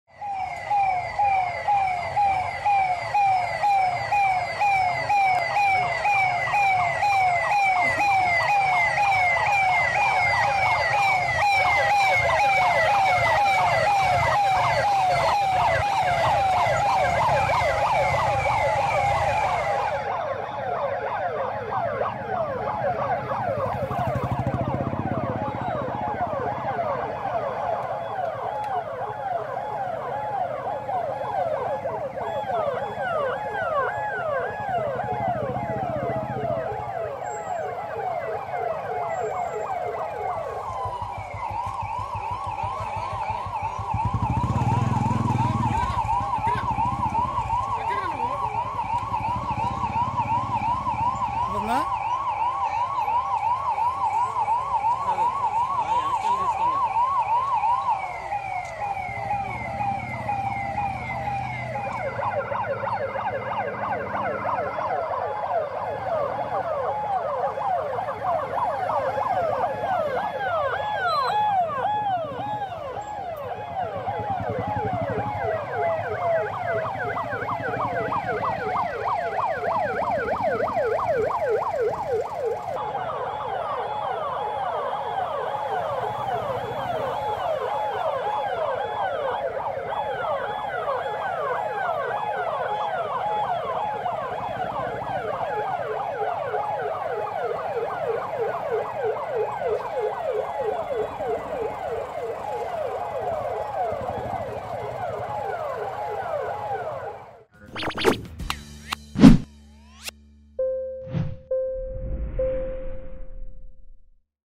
Vijayawada: ప్రజలకు అవగాహన కల్పిస్తూ వాహనాలతో మార్చ్ ఫాస్ట్ నిర్వహించిన పోలీసులు... I News18 Telugu
కృష్ణలంక ప్రాంతాలలో పర్యటించిన కలెక్టర్ ఇంతియాజ్, సిపి ద్వారకా తిరుమలరావు. ప్రజలకు అవగాహన కల్పిస్తూ వాహనాలతో మార్చ్ ఫాస్ట్ నిర్వహించిన పోలీసులు